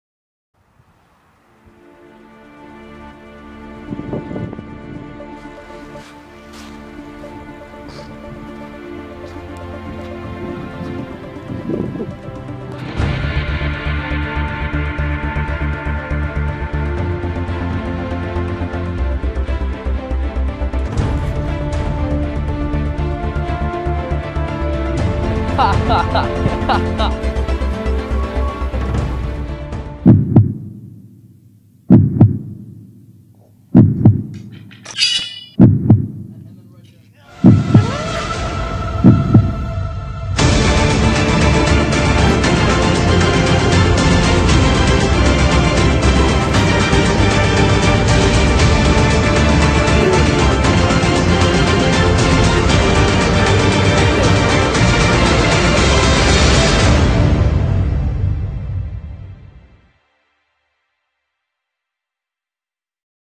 iMovie School Trailer - Le roi Arthur (King Arthur)
An iMovie school trailer on a book that I read, Le roi Arthur (King Arthur)